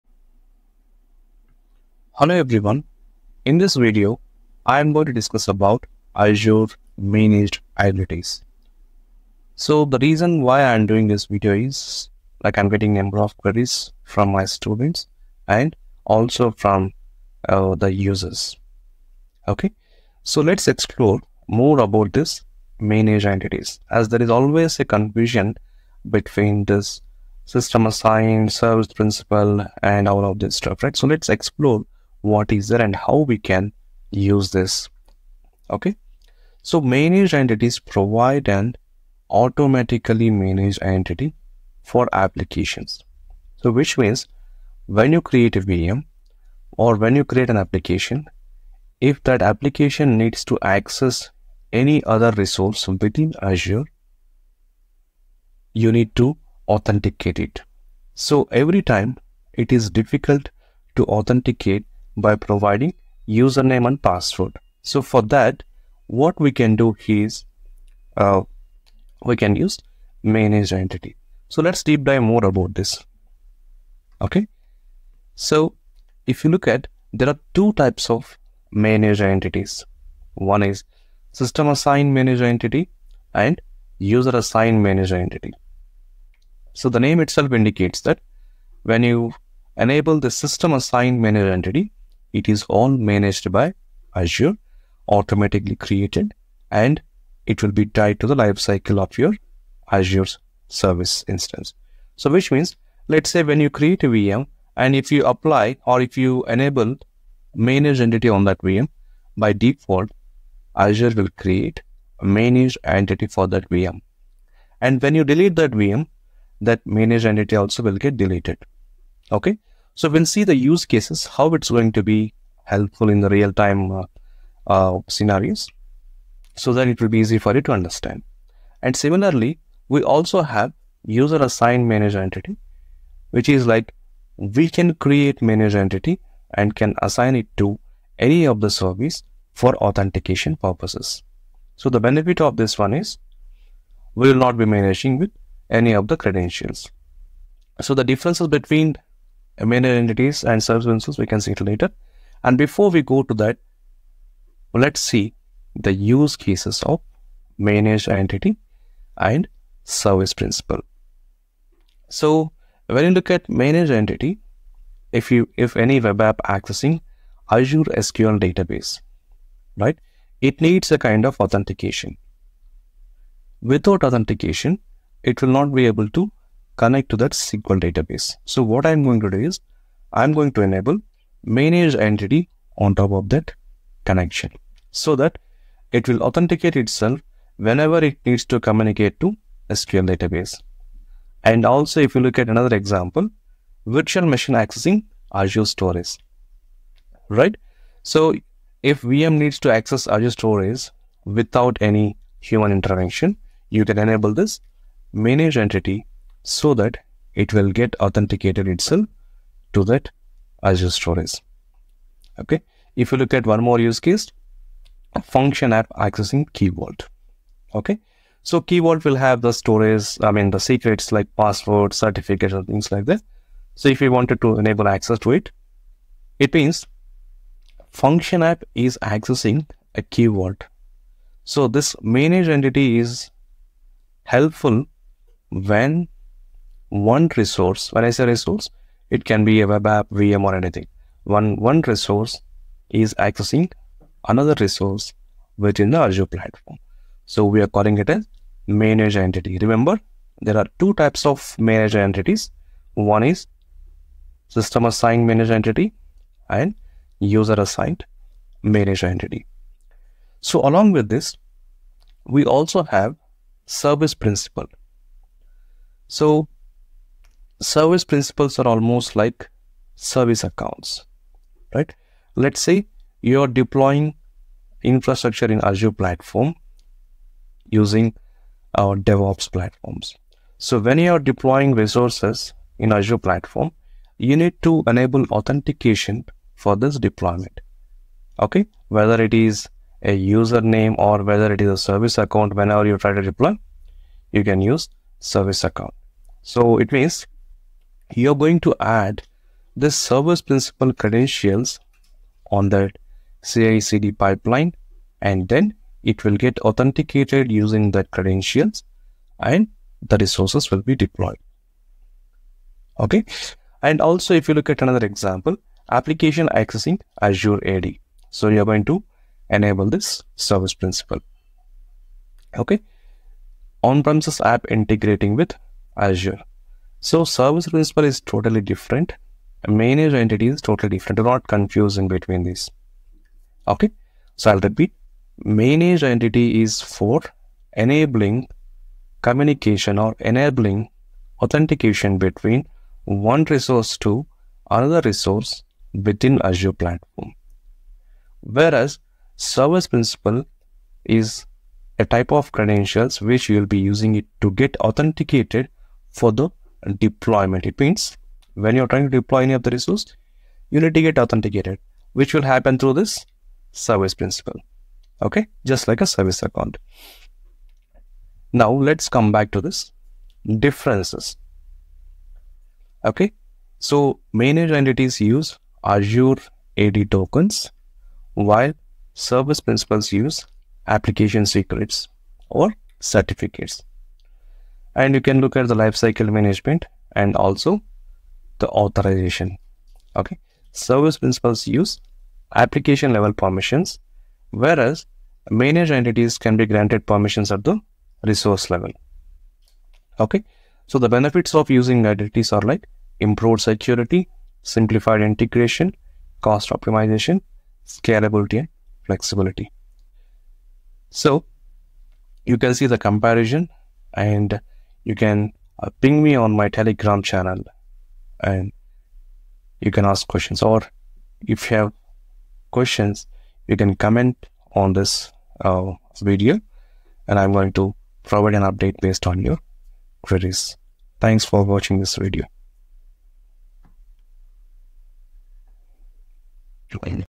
Azure Managed Identity vs. Service Principal: Choosing the Right Authentication Approach | Use Cases
In this video, we’ll unravel the mysteries surrounding Azure Managed Identity. Whether you’re a seasoned Azure enthusiast or just dipping your toes into the cloud waters, understanding managed identities is crucial for secure authentication and seamless resource access.

Remember, whether it’s a silent guardian (managed identity) or a bold trailblazer (service principal), both contribute to a secure Azure environment. Choose wisely, and may your tokens be ever valid! 🚀🔐